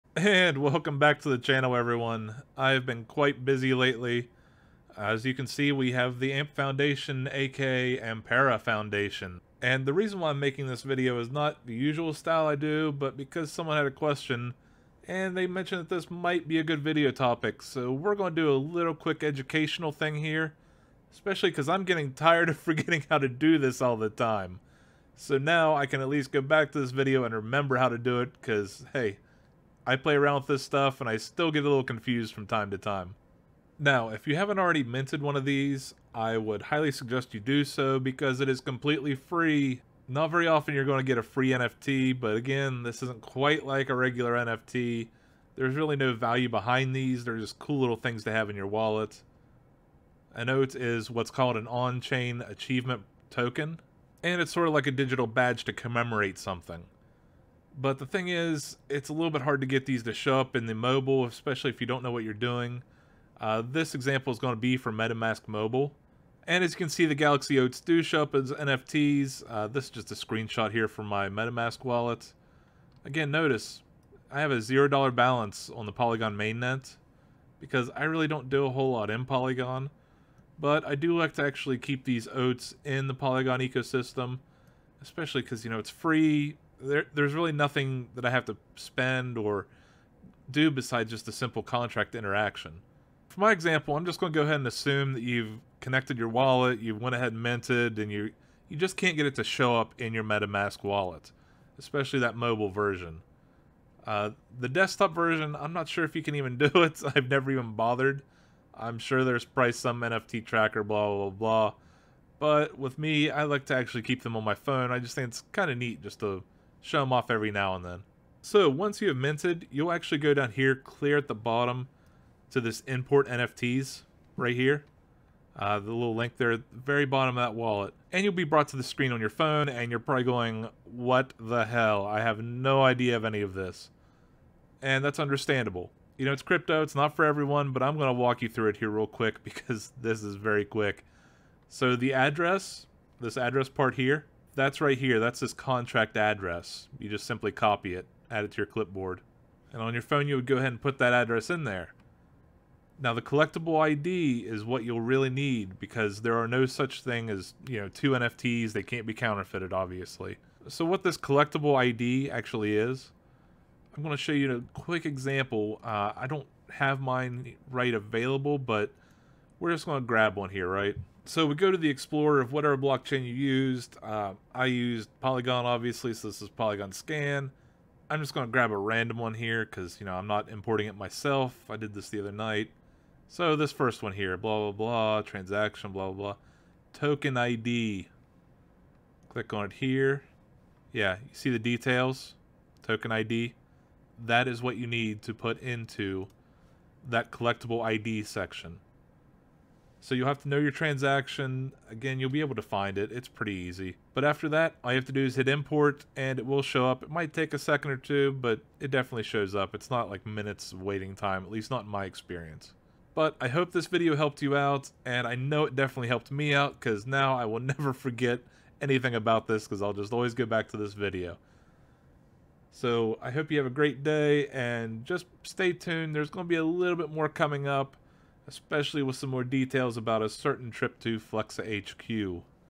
How to get a Galxe / Galaxy OAT NFT to appear on MetaMask mobile.
Pretty short and simple video for those of us who always seem to forget how to do it.